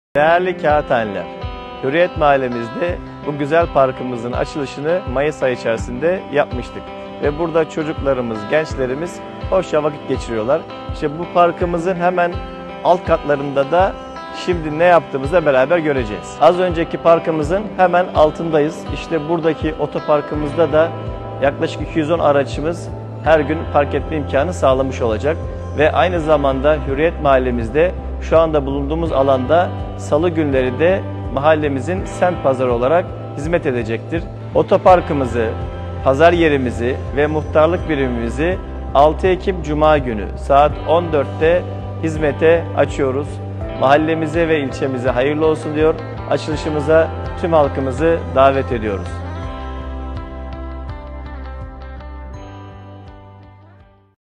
Hürriyet Kapalı Pazar Yeri ve Yer Altı Otoparkımızı Hizmete Açıyoruz
Çocuk parkı, yeşil alanları, 210 araç kapasiteli geniş yer altı otoparkı ve modern kapalı pazar yeri ile ilçemize kazandırdığımız;

🗓️06 Ekim Cuma ⏱️14.00
📍Dr. Cemil Bengü Caddesi